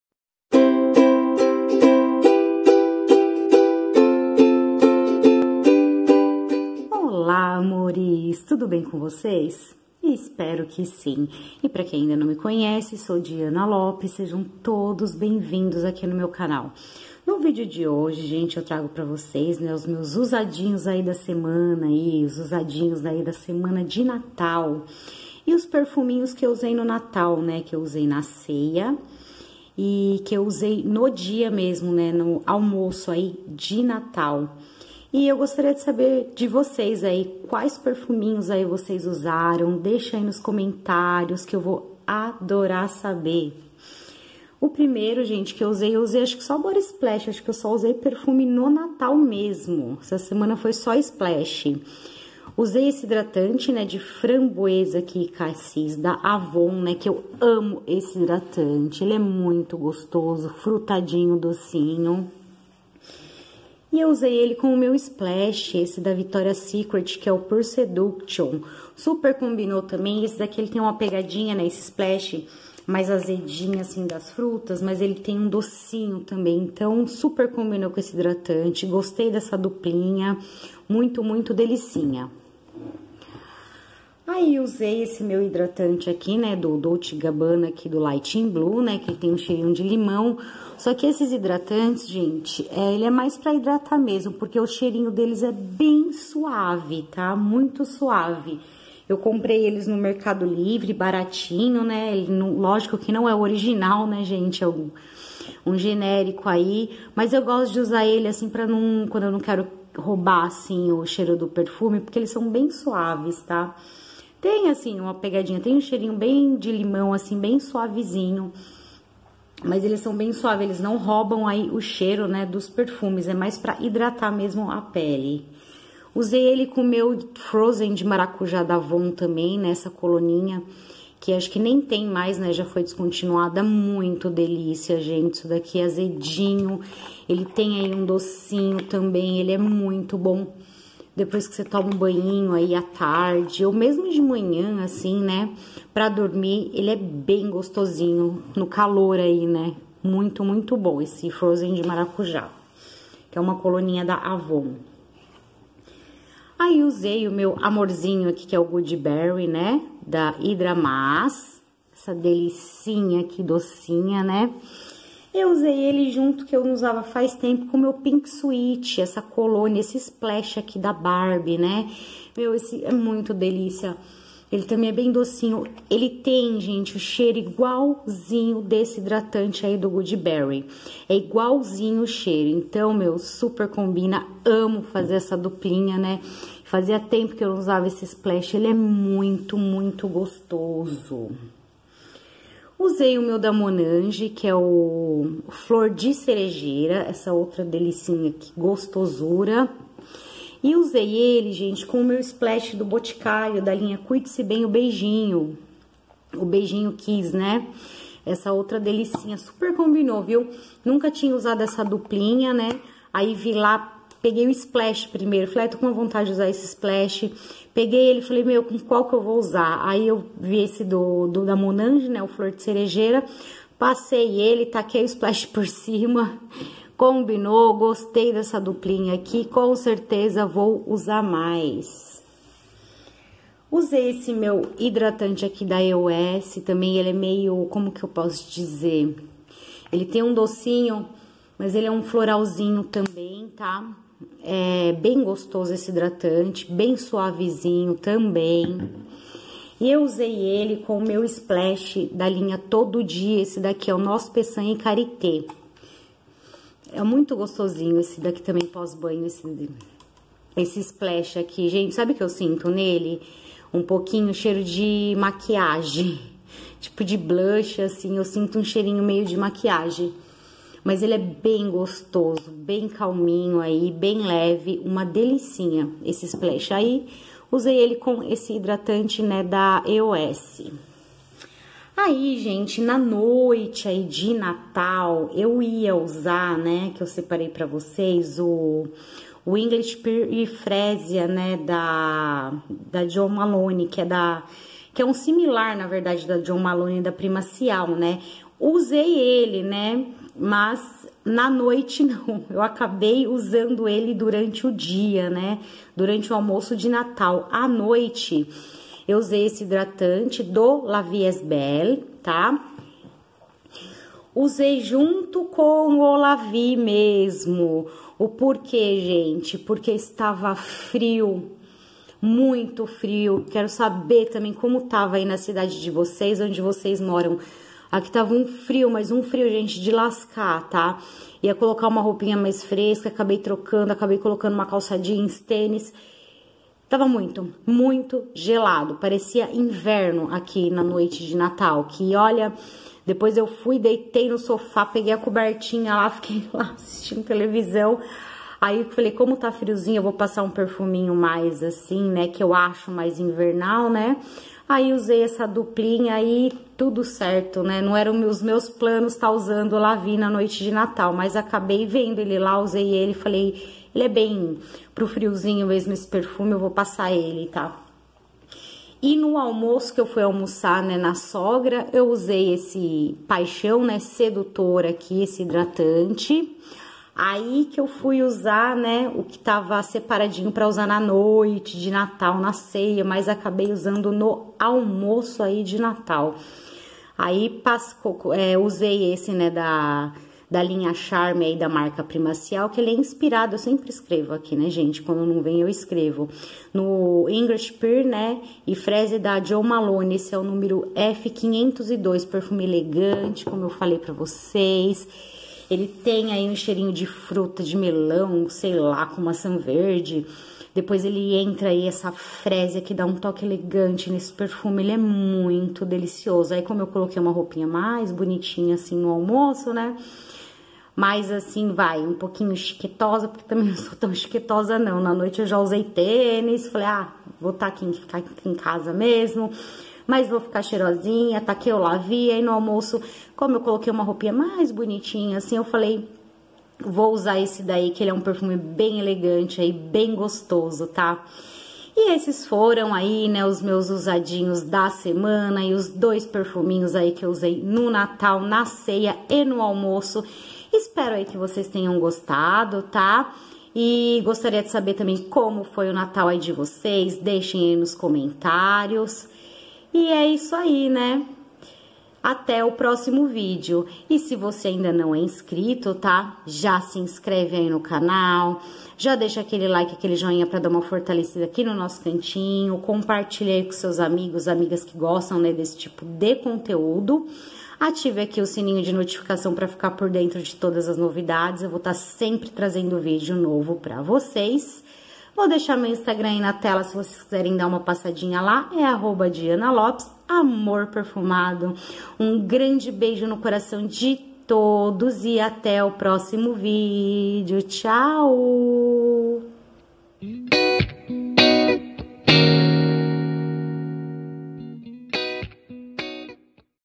Usados da Semana :Perfumes que usei no Natal! 🎅🎄💞
OLÁ AMORES! TUDO BEM COM VOCÊS? ESPERO QUE SIM.
HOJE VOU MOSTRAR TUDO QUE USEI NA SEMANA DE PERFUMES E HIDRATANTES, E OS PERFUMES QUE USEI NO NATAL.

DEIXE NOS COMENTÁRIOS, QUAIS CHEIROSOS VCS USARAM, NA CEIA E ALMOÇO DE NATAL🎅🎄, VOU ADORAR SABER.

SE VC GOSTA DE PERFUMES ESTÁ NO LUGAR CERTO...
GRANDE BEIJO 😘